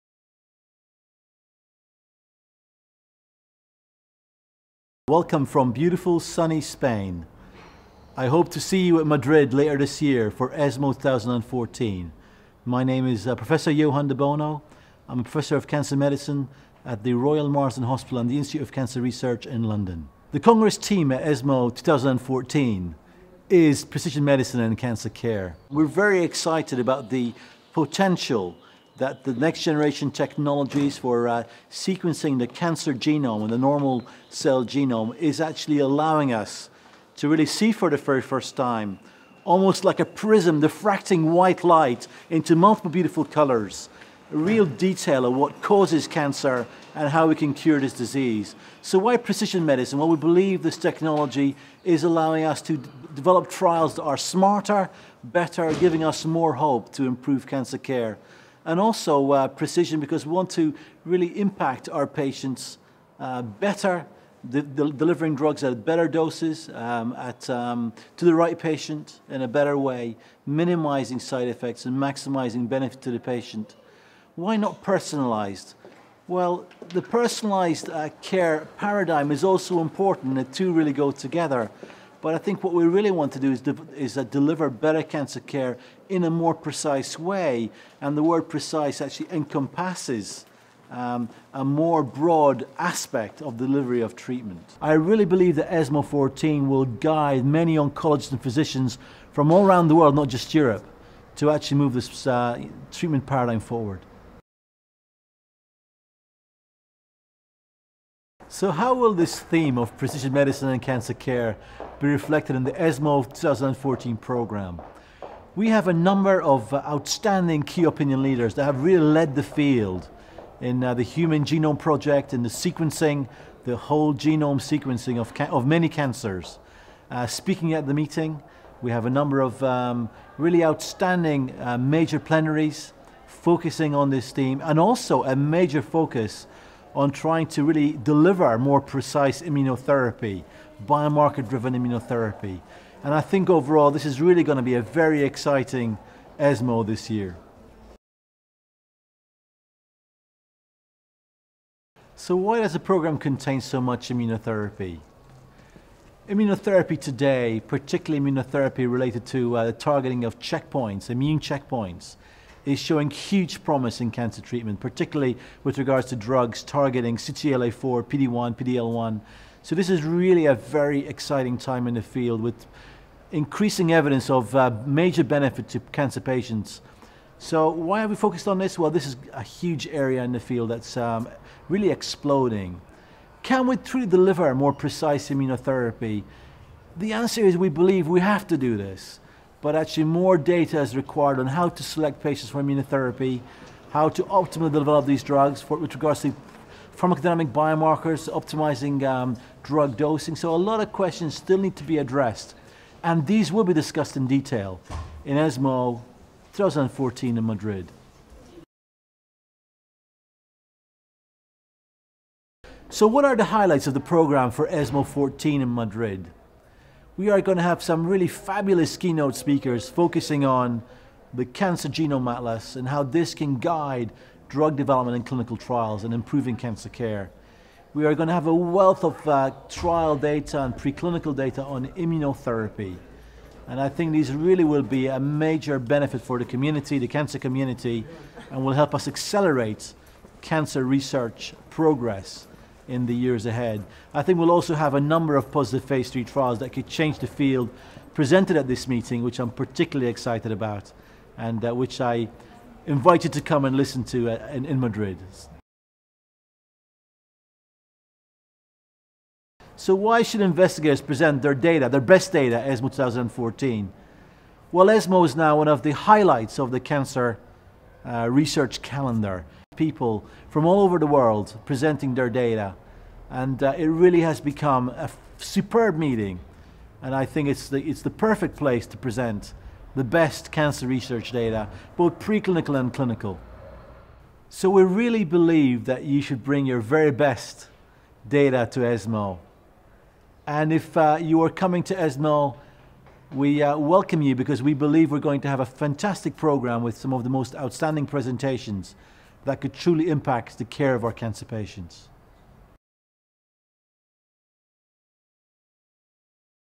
ESMO 2014: Precision medicine in cancer care
J.de Bono, ESMO 2014 Scientific Chair, offers an appetizer of the premier medical oncology congress in Europe, highlighting the high scientific quality of the programme, with world renowned speakers and topics that will enable doctors to improve cancer patient care.

Video produced by the European Society for Medical Oncology (ESMO)